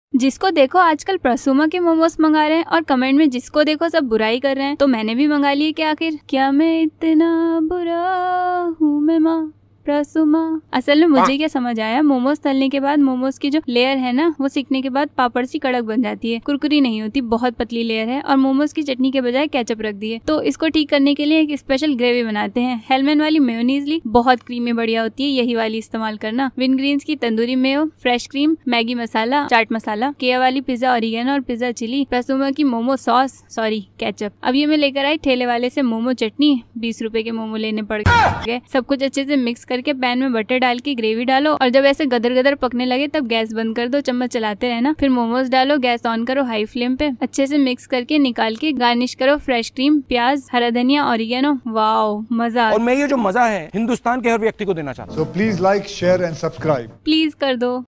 Making Gravy Momos at ₹100 & eating them for the whole day!🤫🤫🤫🤫🤫🤫🤫🤫🤫🤫🤫momos
Ingredients:-
4 Tbsp. Mayonnaise
1 Tbsp. Tandoori Mayonnaise
12 grm. Maggi Masala
1/4th tsp. Chaat masala
Sprinkle some oregano
1 Tbsp. Butter
Chilli flakes
1 Tbsp. Fresh cream
2 packet . Prasuma chutney
4 Tbsp. Street Momos spicy chutney
Onion
Coriander
Fresh cream